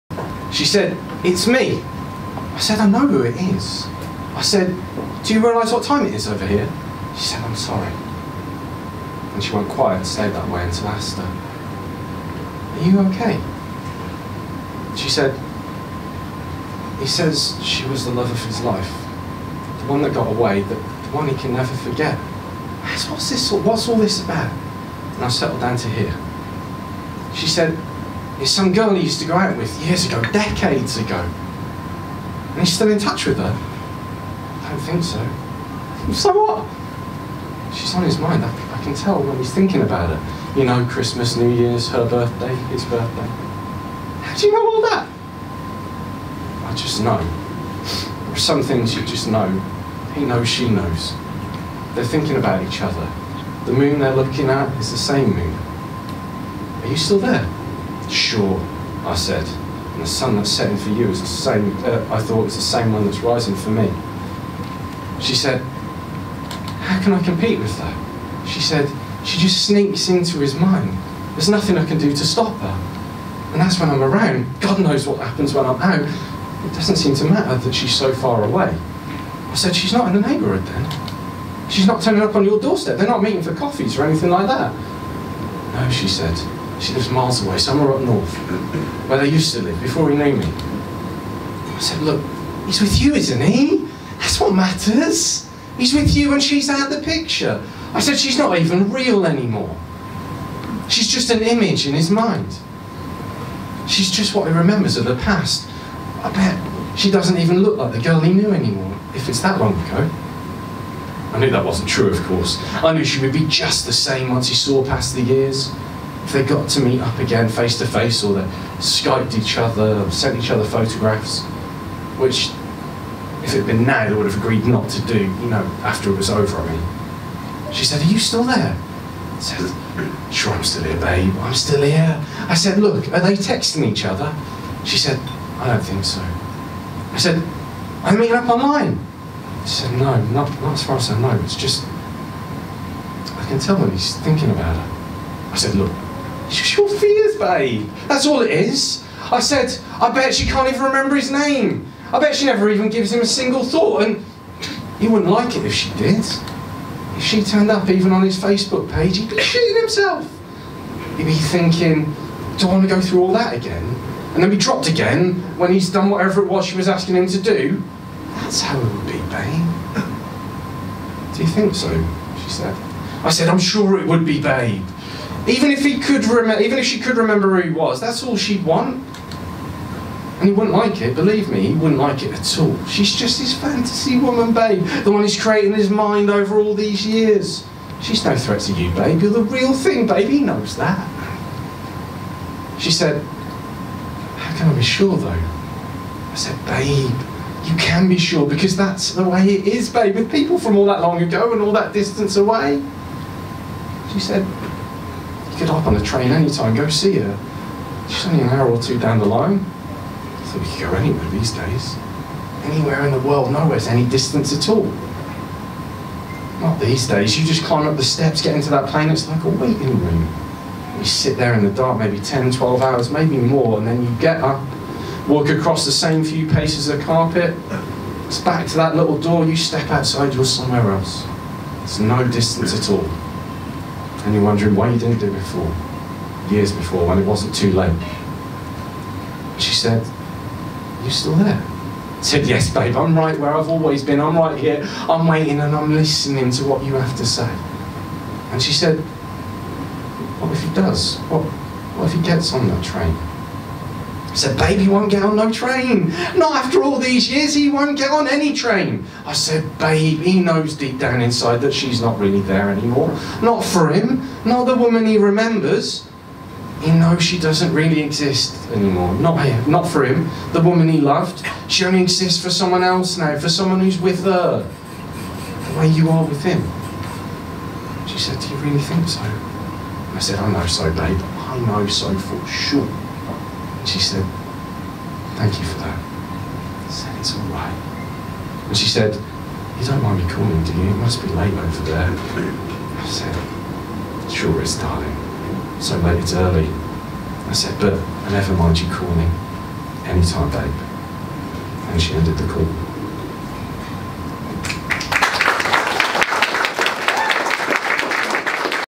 “It's Only Time That Parts Us” by Brindley Hallam Dennis (read by Angus Gallagher)
From the Liars’ League Hong Kong event ‘Near & Far’ held at Social Room in Hong Kong on February 26th, 2018.